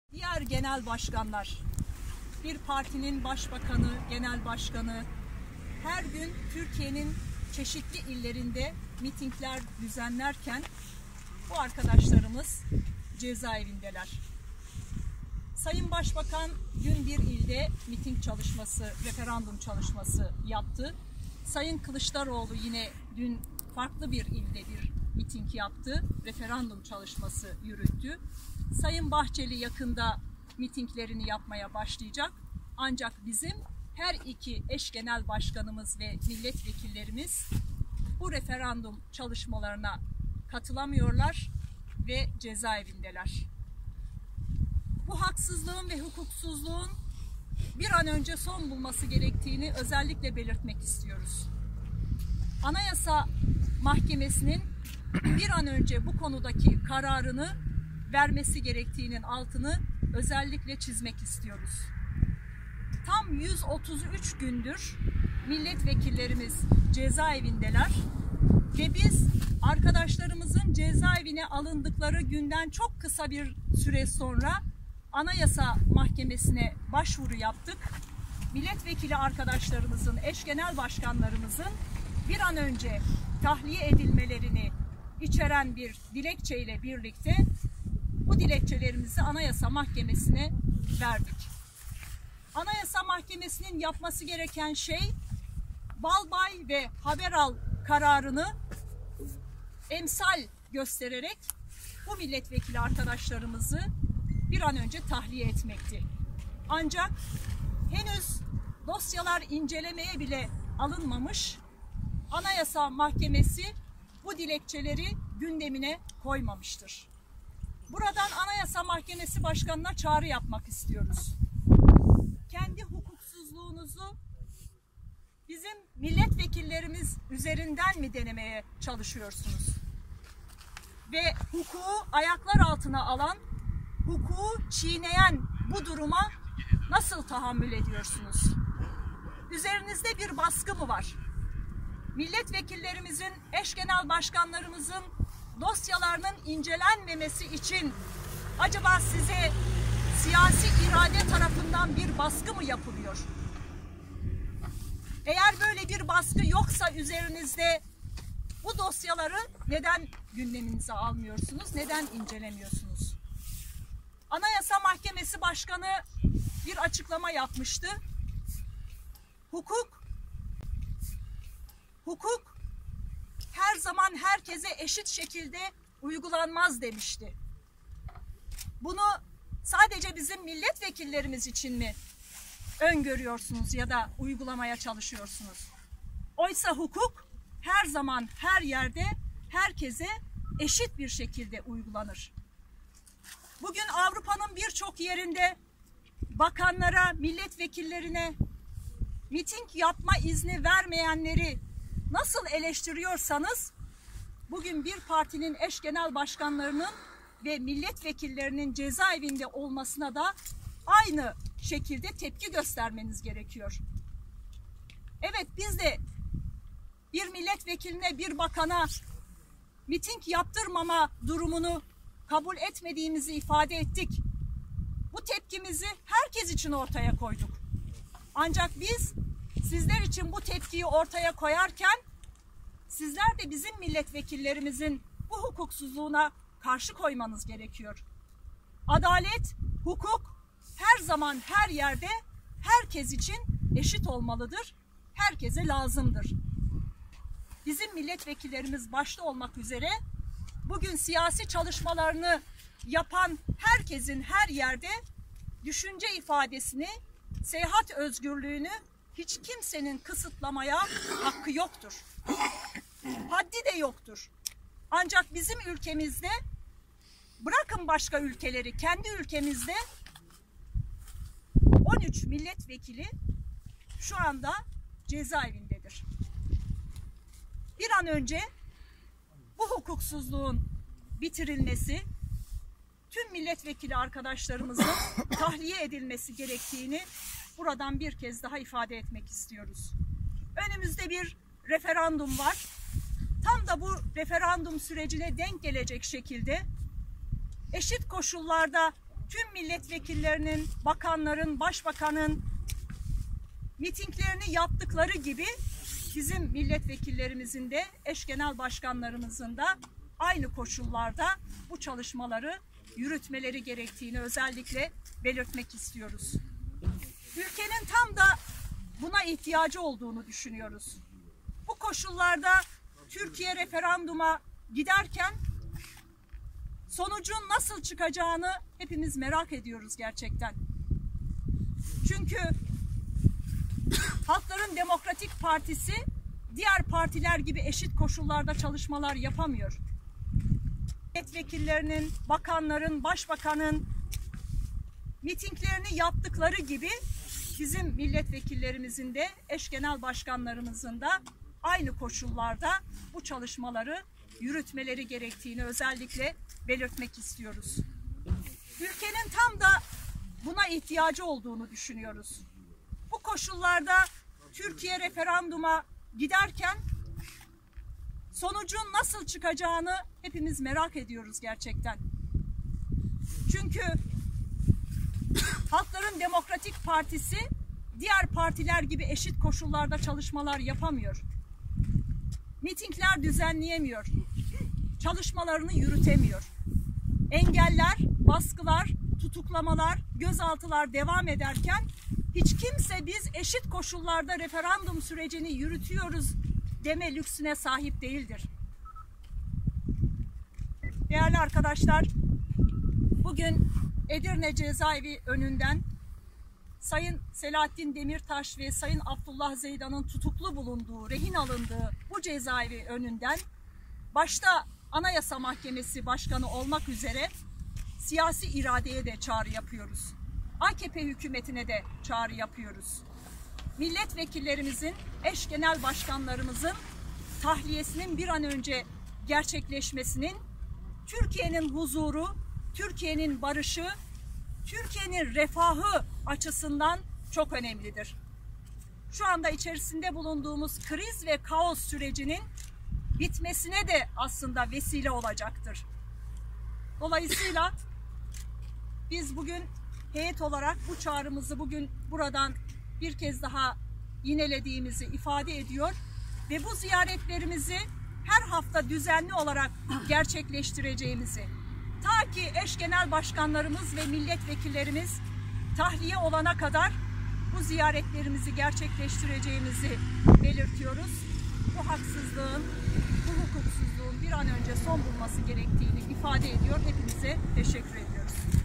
Heyetimiz Edirne Cezaevi'nden AYM'ye seslendi: Üzerinizde baskı mı var?
Eş Genel Başkanımız Selahattin Demirtaş ve Hakkari Milletvekilimiz Abdullah Zeydan'ın tutulduğu Edirne Cezaevi'ne giden heyetimiz, cezaevi önünde açıklama yaptı.